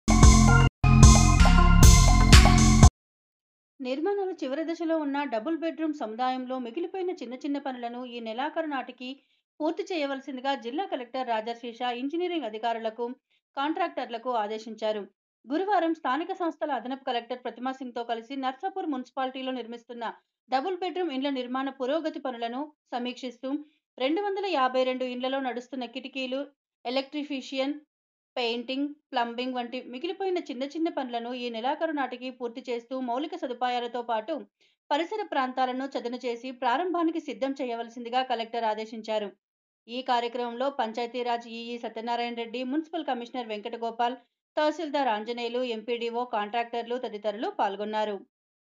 నర్సాపూర్ డబుల్ బెడ్ రూమ్ ఇళ్ల నిర్మాణ పురోగతి పనులను జిల్లా కలెక్టర్ రాజర్షి షా పరిశీలించారు
నర్సాపూర్ డబుల్ బెడ్ రూమ్ ఇళ్ల నిర్మాణ పురోగతి పనులను జిల్లా కలెక్టర్ రాజర్షి షా  అదనపు కలెక్టర్ ప్రతిమ సింగ్ తో కలిసి పరిశీలించారు